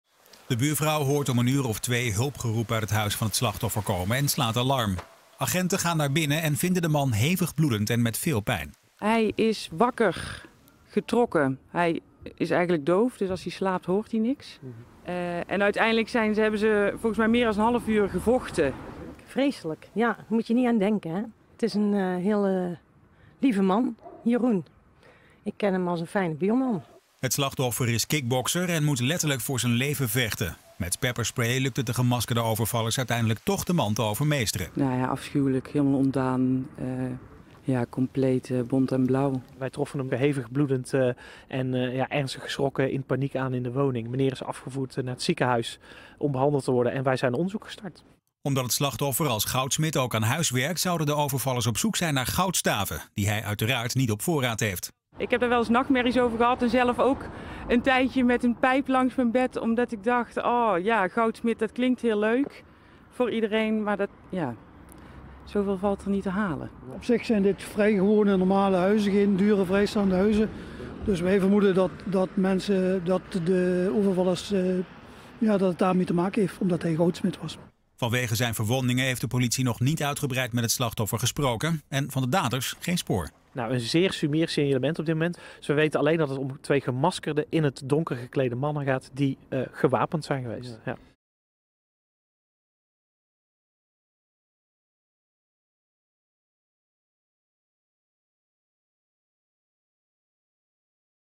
Dove goudsmid moet vechten voor zijn leven
Een dove goudsmid heeft moeten vechten voor zijn leven. De 54-jarige man werd midden in de nacht in zijn eigen huis overvallen, en met een stuk van de trapleuning in elkaar geslagen.